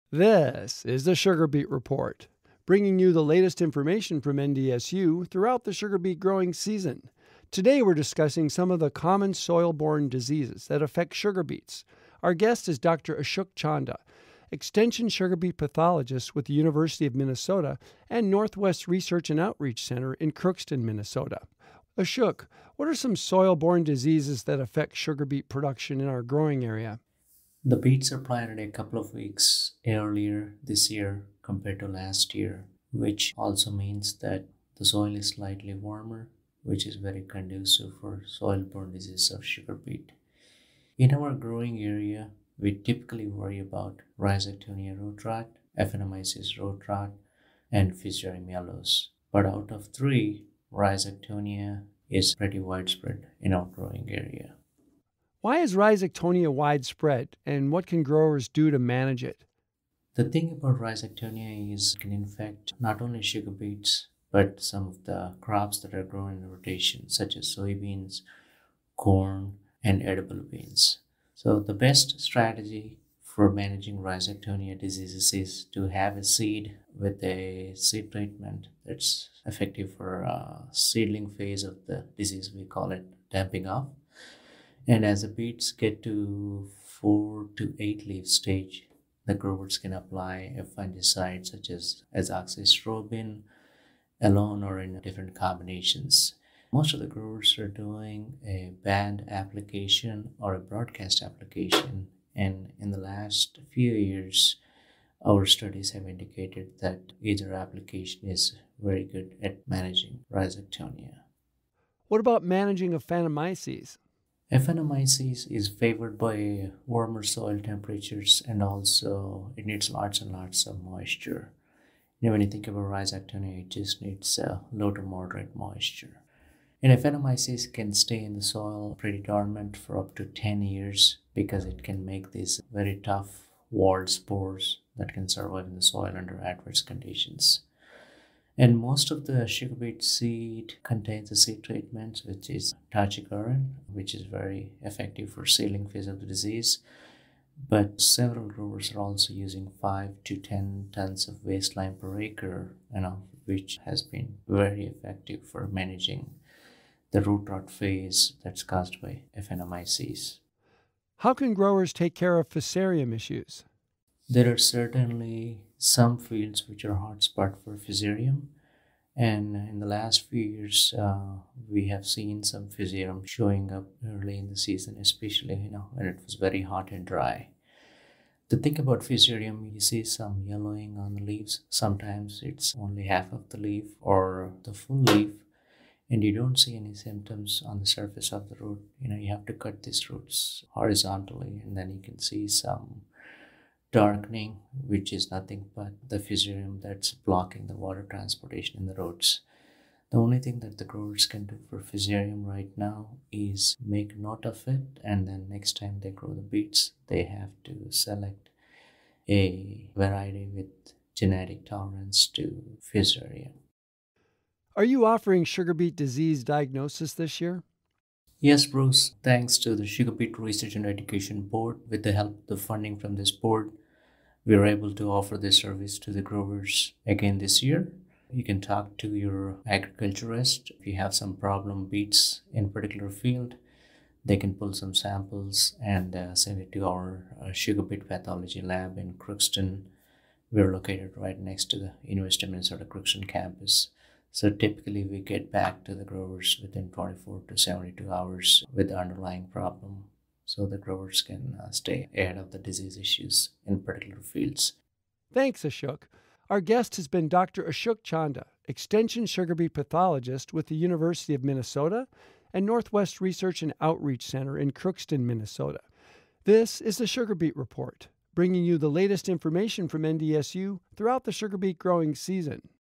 Soilborne Diseases - Sugarbeet Report
Dr. Ashok Chanda, Extension sugarbeet pathologist with the University of Minnesota and Northwest Research and Outreach Center in Crookston, discusses some of the common soil borne diseases that affect sugarbeets.